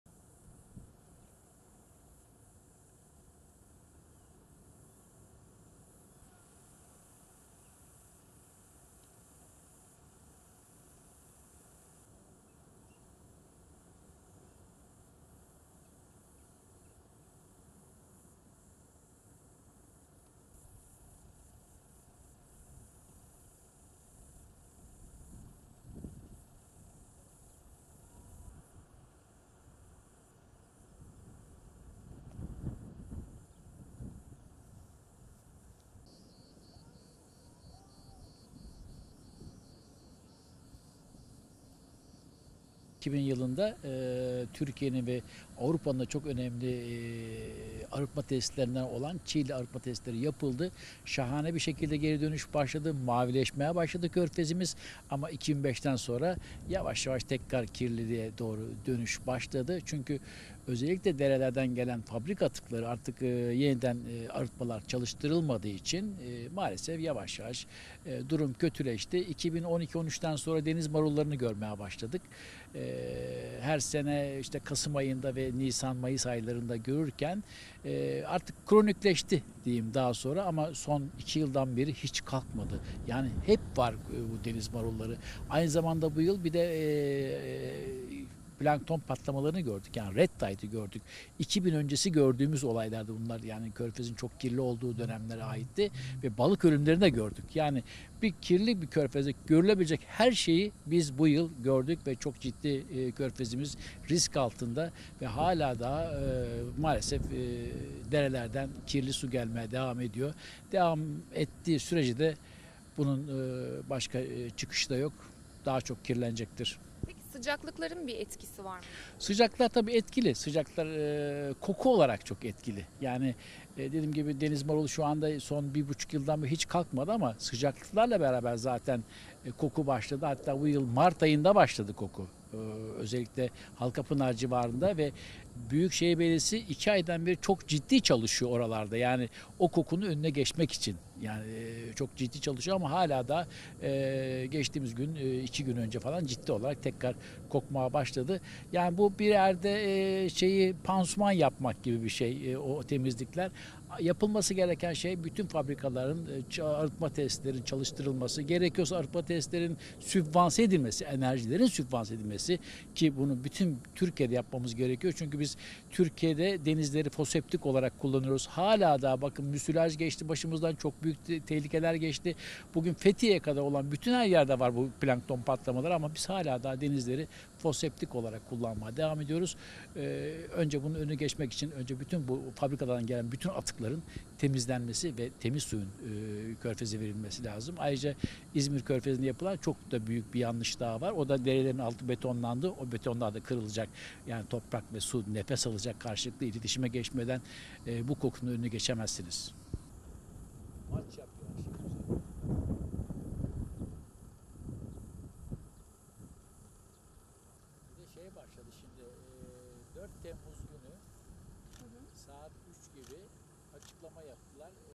İzmir'de Çakalburnu Lagünü'nün 3'te 1'i deniz maruluyla kaplandı
İzmir Körfezi'nde, sıklıkla görülen deniz marulu, hava sıcaklıklarının yükselmesiyle, ciddi oranda arttı. Yüzlerce kuş türüne ev sahipliği yapan İnciraltı'ndaki Çakalburnu Lagünü'nün 3'te 1'i deniz maruluyla kaplandı.

Artan kirlilik nedeniyle İzmir Körfezi'nde sıklıkla görülen ve deniz marulu olarak bilinen makroalgler, havaların ısınmasıyla arttı. İnciraltı'nda yüzlerce kuş türüne ev sahipliği yapan Çakalburnu Lagünü'nün 3'te 1'i insan sağlığı açısından risk oluşturmasa da denizleri tehdit eden deniz maruluyla kaplandı. Körfezde deniz maruluyla, kötü kokunun da arttığını söyleyen İzmir Dokuz Eylül Üniversitesi Deniz Bilimleri ve Teknolojisi Enstitüsü Öğretim Üyesi Prof. Dr. Doğan Yaşar, "Maalesef 2000 yılından sonra denizin en kirli olduğu dönemi yaşıyoruz. 2000 yılında Türkiye'nin ve Avrupa'nın da çok önemli arıtma tesislerinden olan Çiğli Arıtma Tesisi yapıldı. Şahane bir şekilde geri dönüş başladı. Körfezimiz mavileşmeye başladı. 2005 yılından sonra yavaş yavaş tekrar kirliliğe dönüldü. Yavaş yavaş durum kötüleşti. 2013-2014'ten sonra deniz marullarını görmeye başladık" dedi.

'DERELERİN ALTINDAKİ BETON KIRILMADAN KOKU ÖNLENEMEZ'

Her yıl kasım, nisan ve mayıs aylarında görülen deniz marulunun artık kronikleştiğini ifade eden Prof. Dr. Yaşar, "Son 2 yıldır hiç kalkmadı. Bu yıl plankton patlamalarını gördük. Bunlar 2000 yılı öncesi gördüğümüz olaylardır. Balık ölümlerini de gördük. Kirli bir körfezdeki olabilecek her şeyi biz gördük. Körfez risk altında. Derelerden kirli su gelmeye devam ediyor. Sıcaklıklar koku anlamında çok etkili. Sıcaklıklarla beraber bu yıl mart ayında koku başladı. Büyükşehir Belediyesi 2 aydır özellikle Halkapınar civarında kokunun önüne geçmek için çok ciddi çalışıyor. Birkaç gün önce tekrar kokmaya başladı. Bu çalışmalar pansuman yapmak gibidir. Yapılması gereken şey bütün fabrikaların arıtma tesislerinin çalıştırılmasıdır. Biz Türkiye'de hala daha denizleri foseptik olarak kullanmaya devam ediyoruz. Derelerin altı betonlandı. O betonlar kırılacak, toprak ve su nefes alacak. Karşılıklı iletişime geçmeden bu kokunun önüne geçemezsiniz" ifadelerini kullandı.